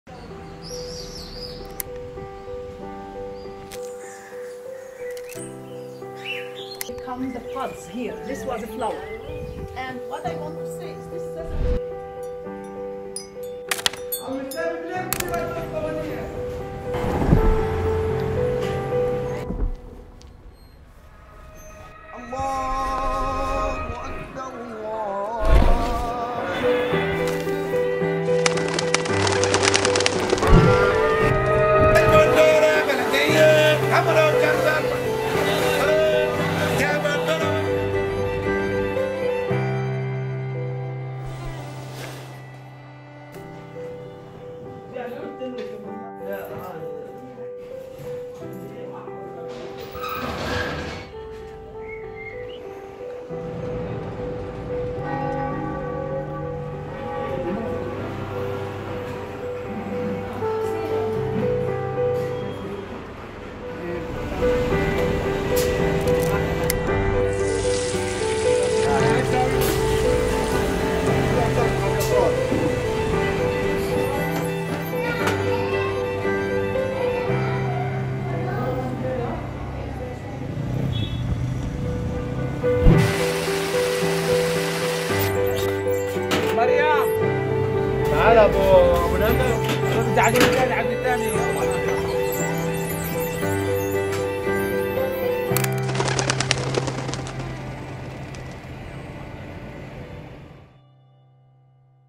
Samsung Galaxy S8+ with DJI Osmo Mobile: Cinematic FHD Video - Ramadan in Nazareth
Walking through the streets of Nazareth on the first day in month of  Holy Ramadan .
All the footage was captured with Galaxy S8+ Camera , while having the Galaxy S8+ mounted on a 3-axis Osmo Mobile gimbal .